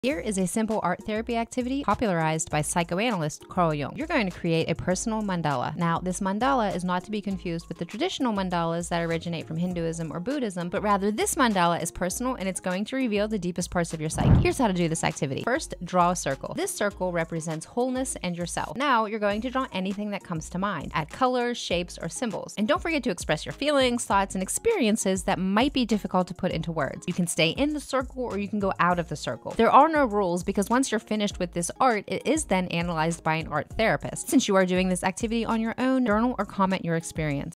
Carl Jung Mandala: EASY art therapy activity 🎨
Here is a simple art therapy activity popularized by psychoanalyst Carl Jung. Youre going to create a personal mandala that reveals your deepest self.

You can create as many as you’d like; they are fun and easy to do!